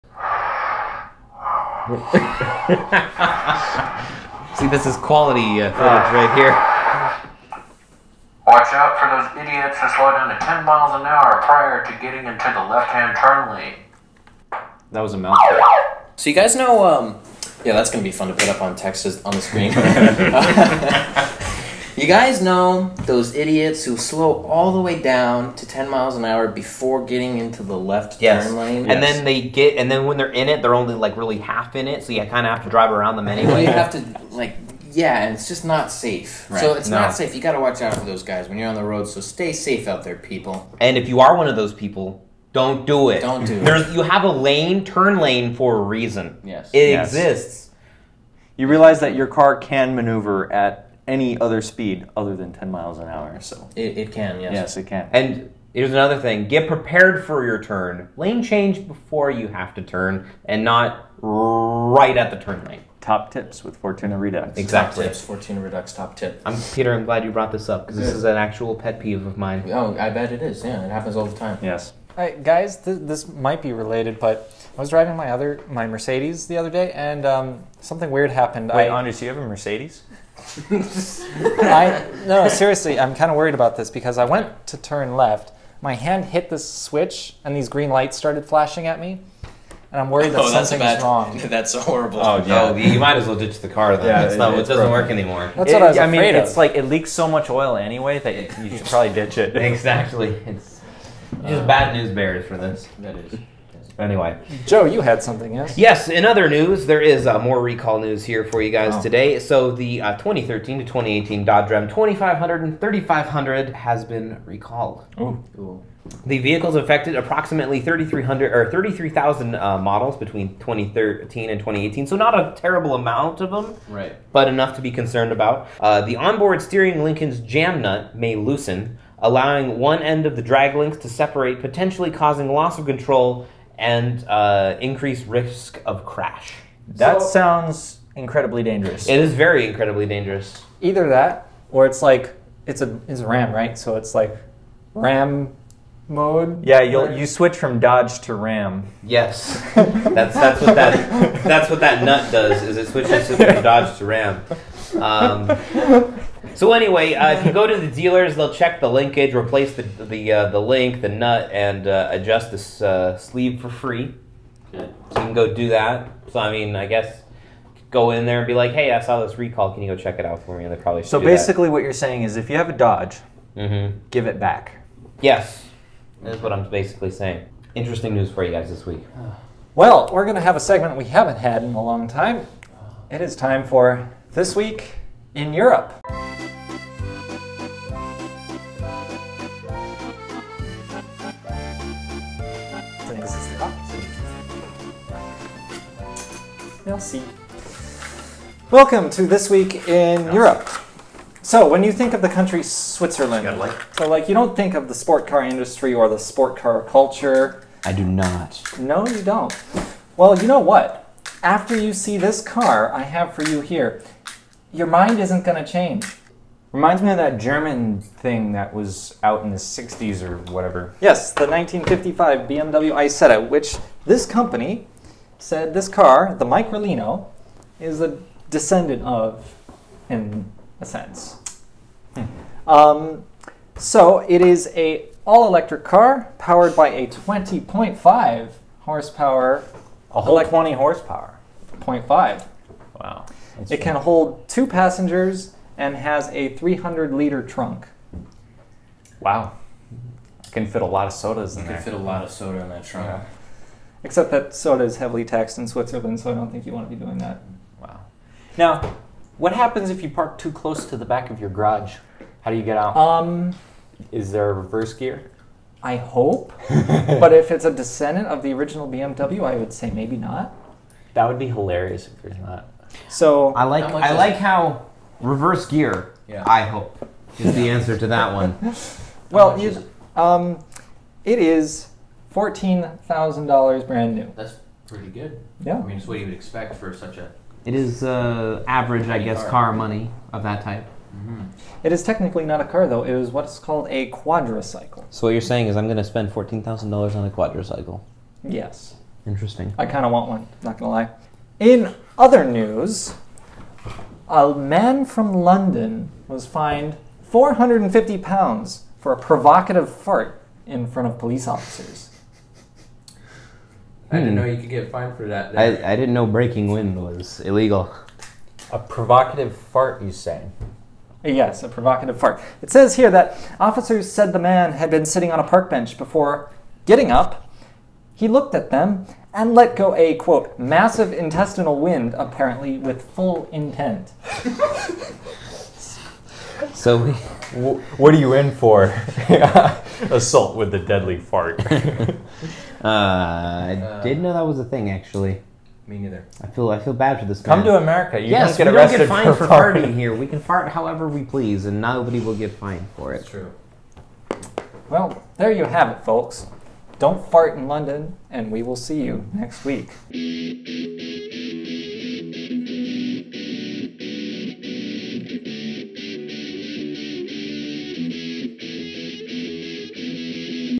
Meet the Microlino | Pete's PSA | Recall Report
This week we talk about the Microlino, a swiss car that is entirely electric and very small. See timecode below for more.

0:00 - 1:47 Pete's PSA
1:47 - 3:00 Recall Report
3:00 - 3:21 Welcome to this week in Europe
3:21 - 5:01 the Microlino
5:01 - 6:11 In other news...